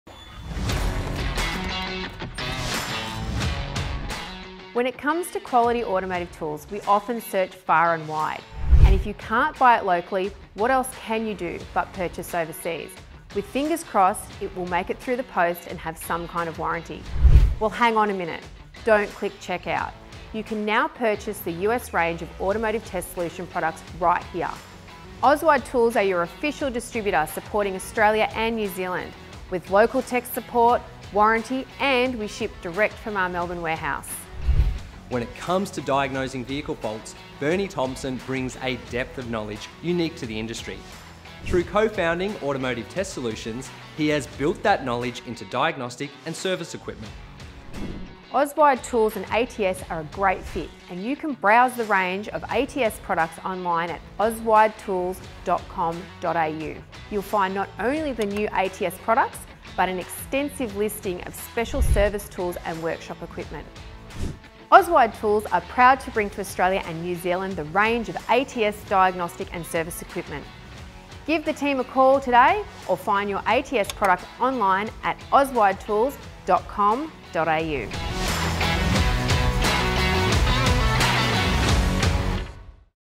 OzWide Tools brings Automotive Test Solutions to Australia and New Zealand
ozwidetools.com.au is proudly the authorized distributor of ATS – Automotive Test Solutions.

ATS has developed a unique arrangement of equipment, to better diagnose and repair modern automotive systems.
Supplying special service tools and advice, OzWide tools is a perfect destination for the ATS product, providing direct technical support, product adaptions and training.